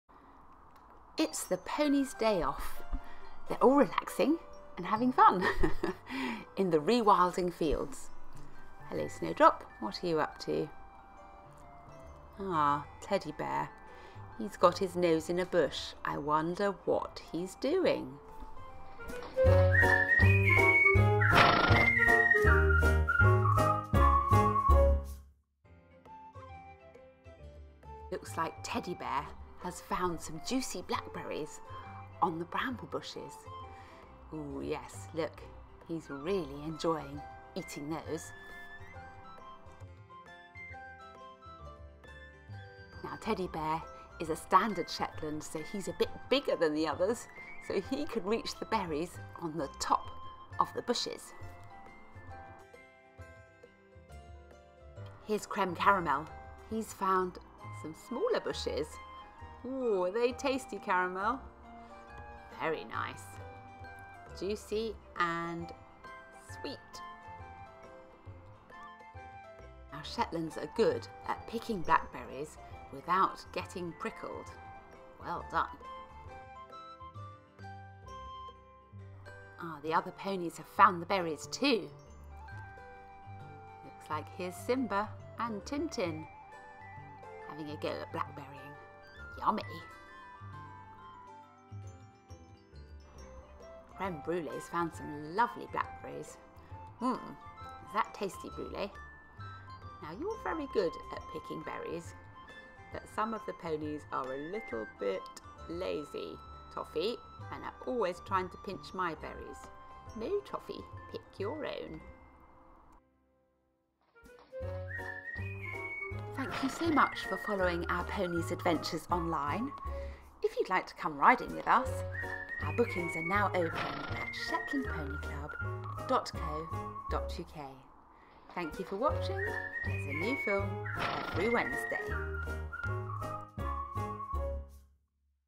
Shetland Ponies eating Blackberries! TV Episode 296
Blackberries are good for Shetland ponies giving them vitamins and they love them .The Shetlands are good at seeking out the juiciest berries and most of our ponies enjoy picking their own blackberries from the bushes... but there is one who is a lazy bones and likes to pinch berries that have all ready been picked...watch the video to find out who it is! involved with Shetland ponies – find out more: